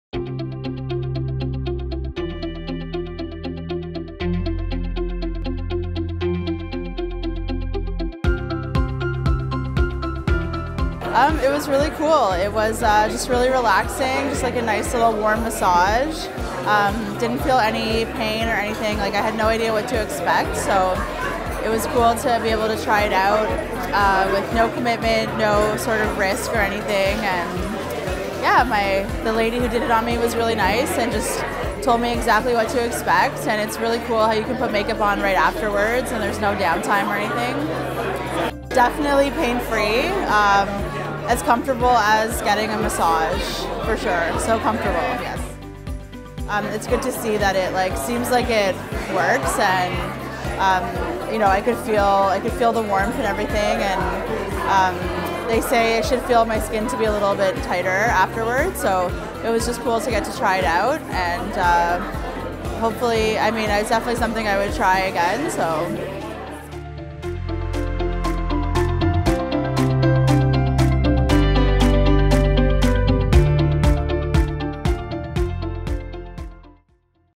סרט עדות - ונוס לגאסי- לקוחה מרוצה בקה לימרה
בקה לימרה היא בלוגרית שעברה טיפול בונוס לגאסי והיא מספרת איך הטיפול היה נעים ומרגיע.